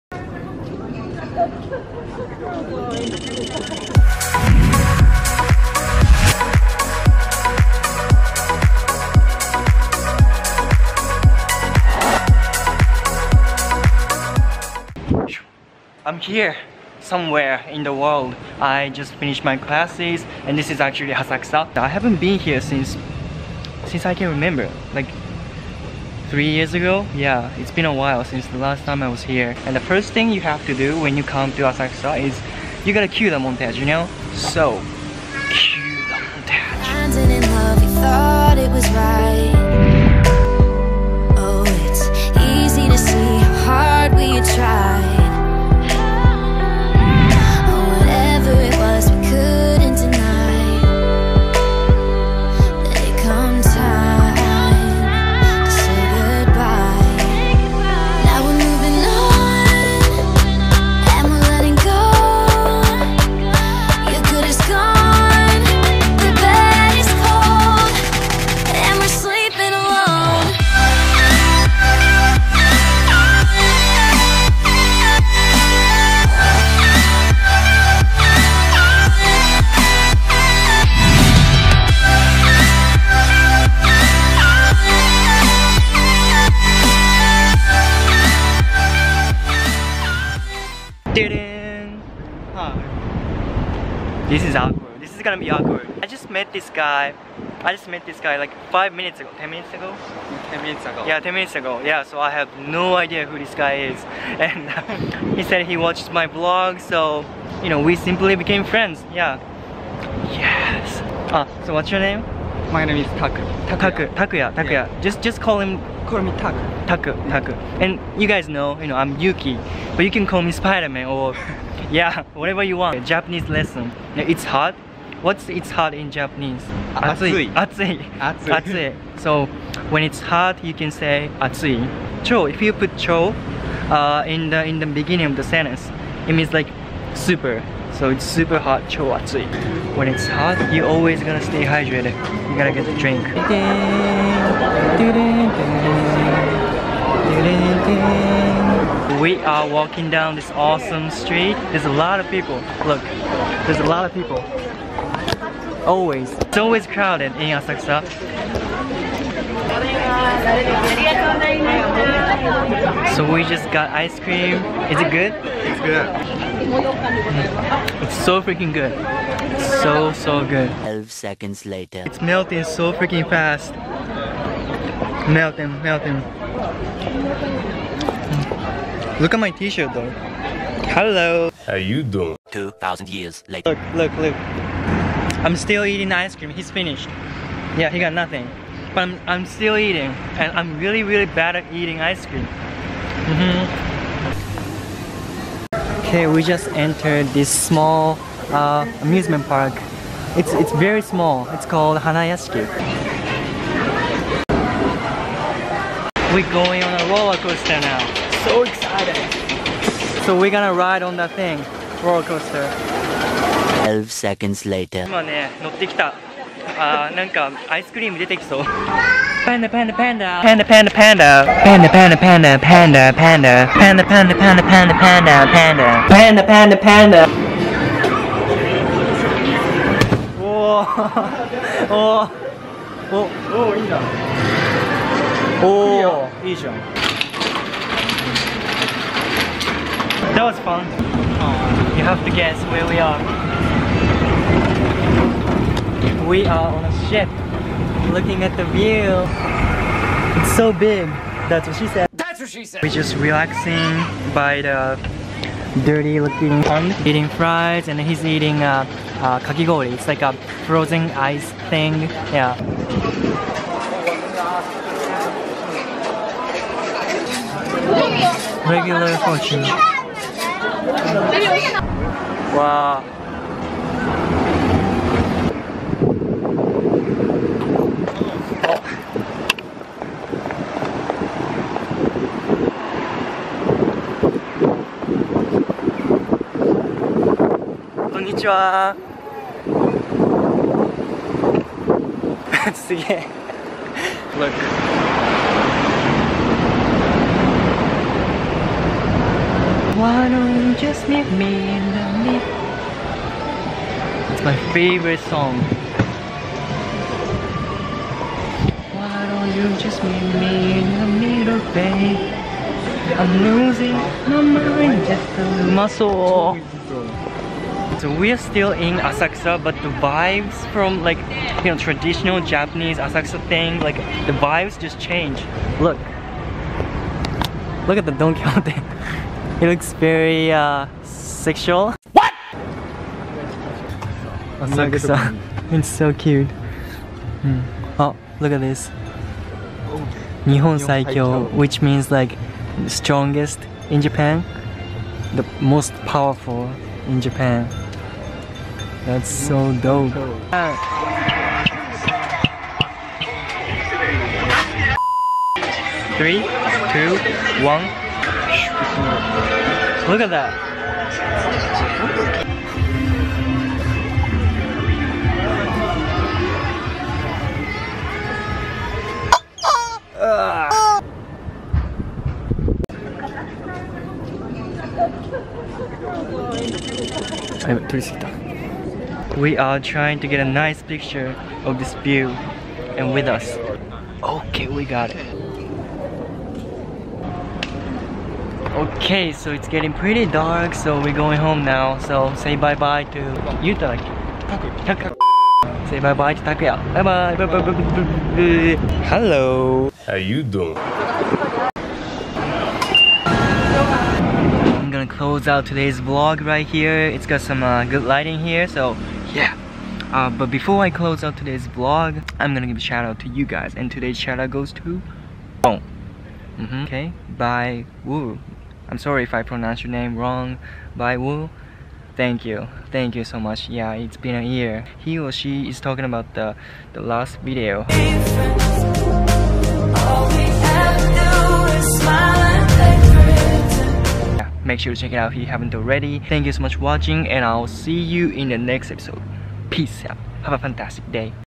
HANGING OUT WITH A VIEWER IN ASAKUSA, TOKYO!
I met a viewer in Asakusa, Tokyo and I think creator on youtube and viewers should feel close to each other sometimes you know? so we hung out! I really enjoyed editing the montage in this vlog so I hope you liked it as much as I did! I haven't been to Asakusa for a couple of years so it was cool to walk around there and get to see the famous temple like Sensoji! Also Japan's oldest amusement park Hanayashiki was great as well!

I'm a Japanese Vlogger from Tokyo and I vlog my life!